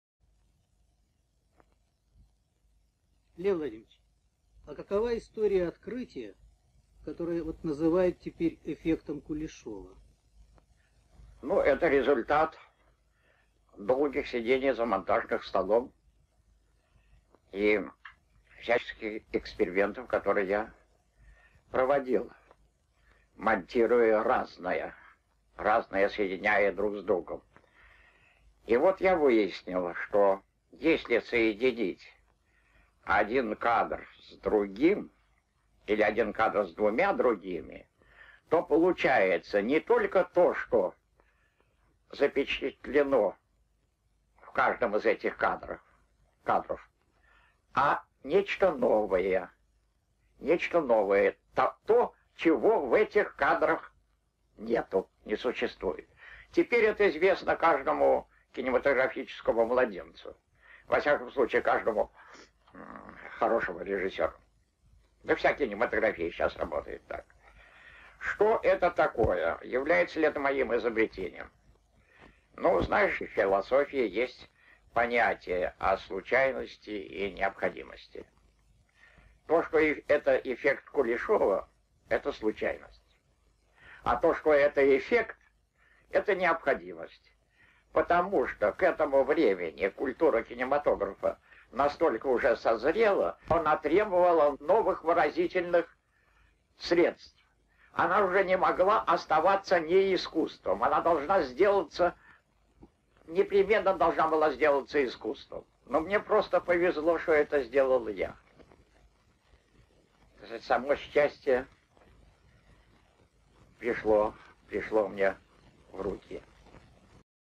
Lev Kuleşov: Kuleşov Efekti'ni Anlatıyor (1969)
Dünyanın ilk film okulu olan Moskova Film Okulu'nun kurucularından biri olan ve aynı zamanda Rus ve Sovyet film yapımcısı ve film teorisyeni olan Lev Kuleşov, özellikle filmciliğin teknik boyutlarına katkılarıyla bilinmektedir.

Kuleşov'un sinema dünyasına en büyük katkılarından biri de hiç kuşku yok ki "Kuleşov Efekti"dir. Buna göre, iki farklı kare bir araya getirilince söz konusu karelerde bulunmayan bambaşka bir anlamın doğmasına sebep olur. Kuleşov, kendisiyle yapılan 1969 tarihli bir röportajda, bu efektin sinemanın ilerleyiş yolunca bir gereklilik ve zorunluluk olduğunu, böyle bir efekti kendisinin keşfetmiş olmasının ise bir şans olduğunu ifade ediyor.

Geçen yıl yaptığım 1964 tarihli bir Alfred Hitchcock çevirinde, Alfred Hitchcock bizzat bu efekti anlatmıştı. O videoyu izlemek isteyenler de buradan ona ulaşabilirler

Kuleşov Efekti'ni biraz daha somutlaştırmak adına Alfred Hitchcock'tan alıntı yapayım:

"Filmin kurgulanması ve farklı bir kanı oluşturmak için nasıl değiştirilebileceğiyle ilgilidir bu. Önce adama yakın çekim yapılır, sonra ise onun ne gördüğü gösterilir. Diyelim ki kucağında bebek tutan bir kadını gördünüz. Hemen ardından adamın gördüğüne nasıl tepki verdiğine bakıyoruz: Gülümsüyor. Öyleyse adamın karakteri nasıldır? Kibar biridir. Sempatiktir.

Pekala, şimdi çocuklu kadının olduğu kısmı filmden çıkaralım ama adamın olduğu iki kısım olduğu gibi dursun. Çıkan kısım yerine bikinili bir kız koyalım filme. Adam bakıyor, bikinili kız var, adam gülümsüyor. Adam şimdi nasıl biridir? Kart bir zamparadır. Artık bebekleri seven iyi yürekli bir beyefendi değildir.

İşte film sizi böyle etkileyebilir."